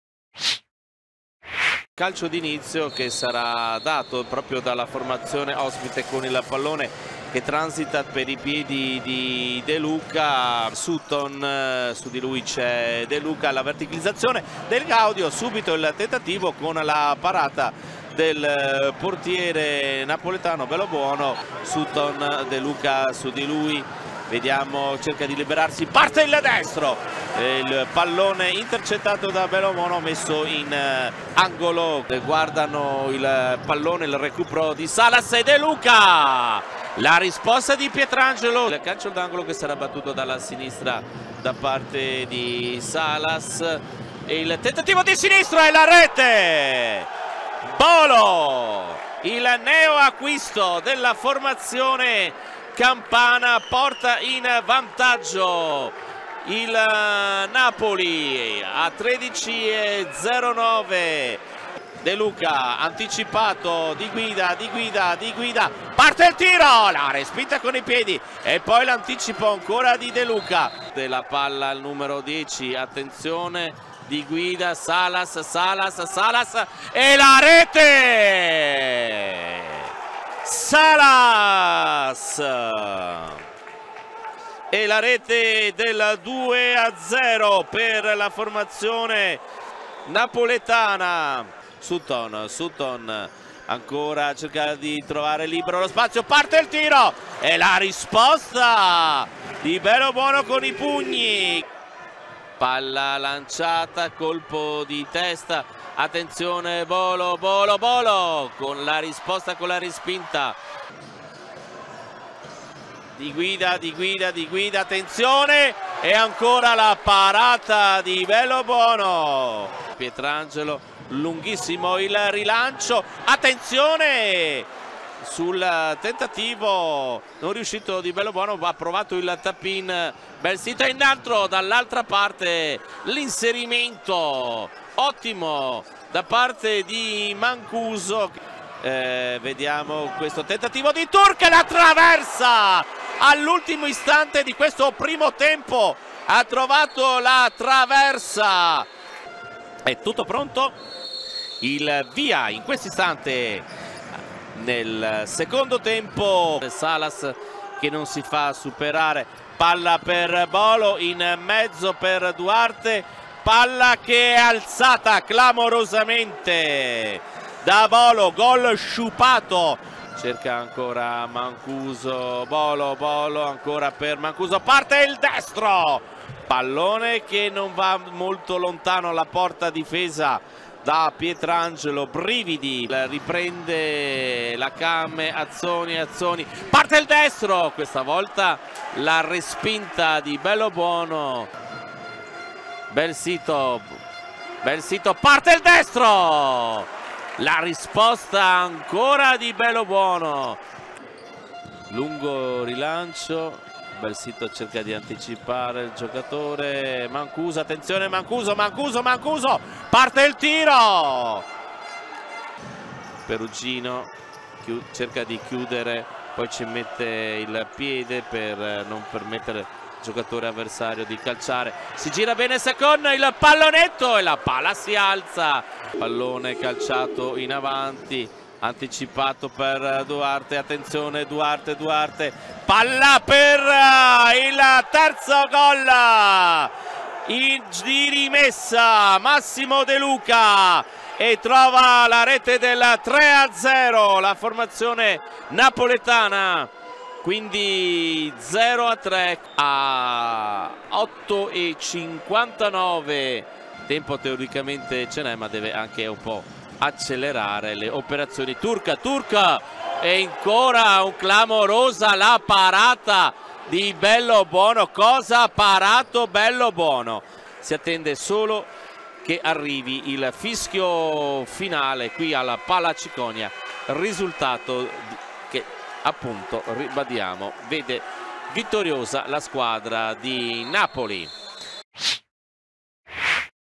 FUTSAL SERIE A, HIGHLIGHTS CAME TREVISO-NAPOLI FUTSAL: LA SESTA SINFONIA FUORICASA💙
Il Napoli si conferma squadra da trasferta, sesto successo, gli azzurri vincono ancora e salgono momentaneamente al quarto posto in classifica. Una gara impeccabile da parte dei partenopei: attenti e ordinati in difesa, cinici e spietati in attacco. Un super Bellobuono, con diversi interventi importanti, nega più volte la gioia del gol agli avversari confermandosi tra i migliori portieri nel panorama italiano. Il 2023 degli azzurri, dunque, si conclude nei migliori dei modi. Non hanno preso parte alla gara Colletta, Borruto e Pesk tra le fila dei partenopei, Vieira, invece, per la Came Treviso.